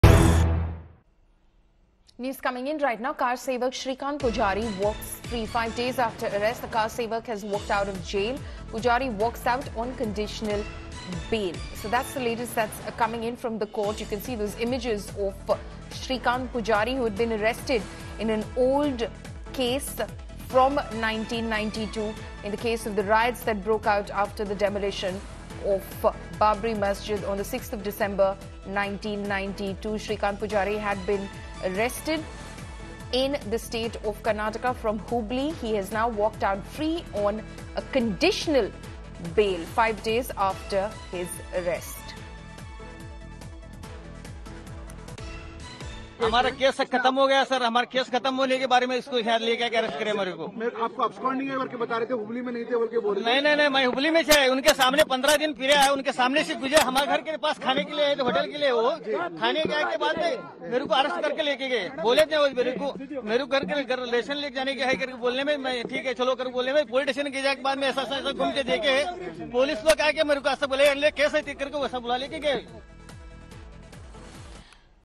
Srikant Poojary Granted Conditional Bail After Five Days | India Today
In a significant development, Karsevak Srikant Poojary Granted Conditional Bail After Five Days has been released on conditional bail five days after his arrest. Shrikant Pujari, a key figure involved in the riots that followed the demolition of the Babri Masjid in December 1992, had been arrested in Hubli, Karnataka. He had remained under custody for an old case that dated back to the year 1992. However, today, Pujari set foot outside the prison walls after being granted a conditional bail by the court. This incident has once again brought the spotlight back onto the haunting incidents of 1992 and the controversial role of key figures like Pujari on that fateful day. KarsevakSrikantPoojary Pujari Granted Conditional Bail After Five Days,